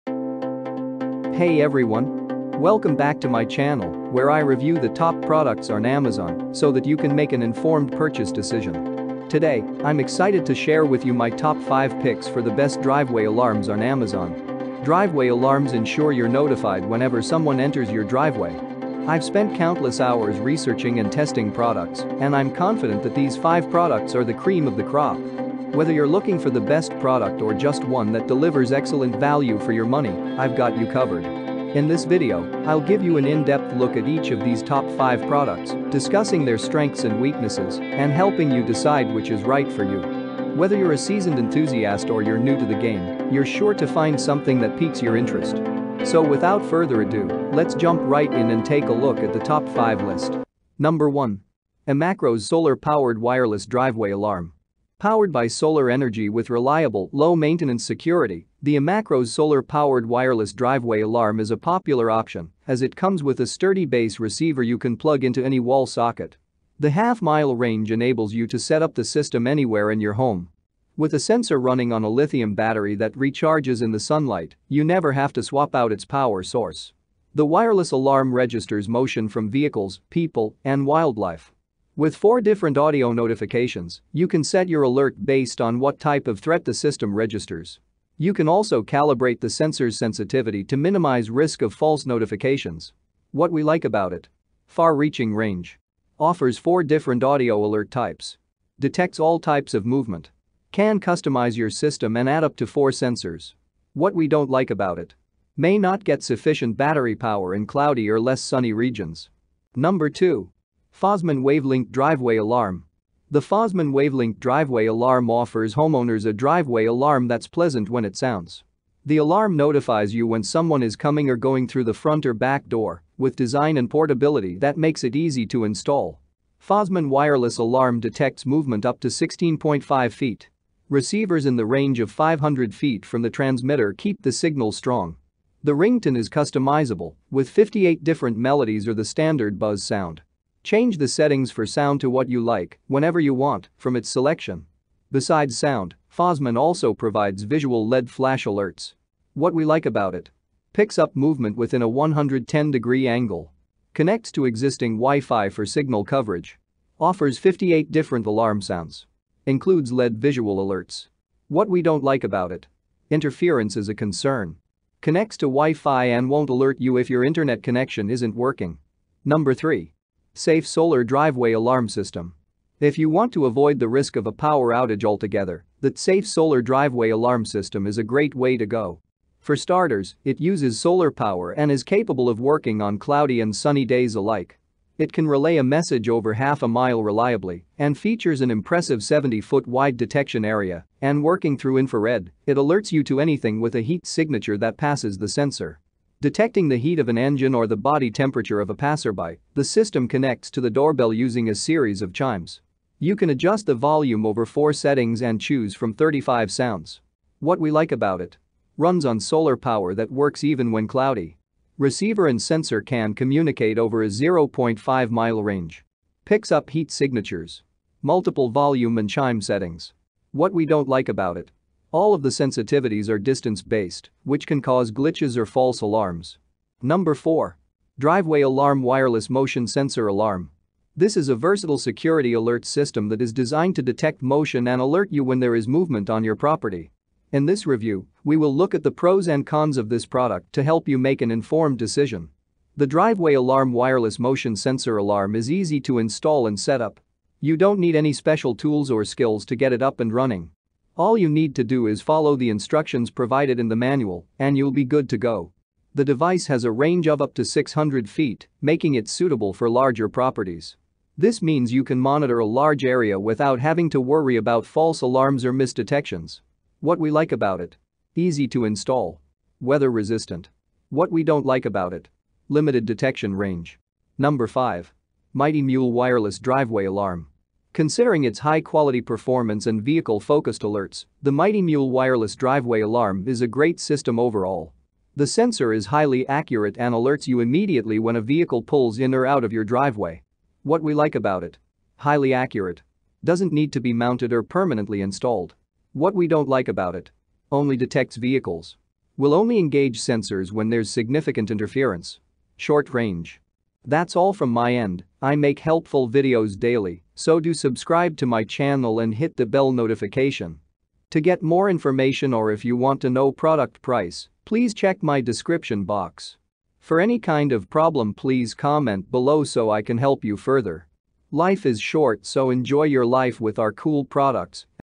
Best Driveway Alarms 2023
📌Product Link📌
✅ My Picks ✅1.eMacros Solar-Powered Wireless Driveway Alarm
✅ Best Value ✅2.Fosmon WaveLink Driveway Alarm
✅ For Beginners  ✅3.HTZSafe Solar Driveway Alarm System
✅ Premium ✅4.Driveway Alarm Wireless Motion Sensor Alarm
✅ Trending  ✅5.Mighty Mule Wireless Driveway Alarm